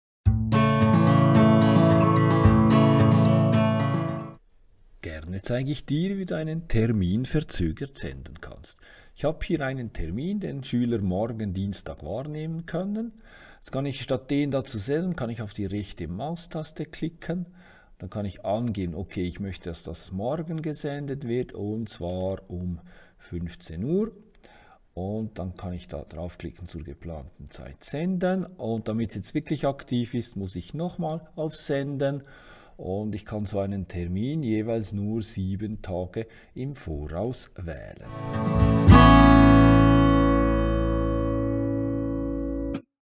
Teams Nachricht verzögert senden
Hier zeige ich dir, wie du eine Nachricht 7 Tage im voraus schon "verzögert" absenden kannst.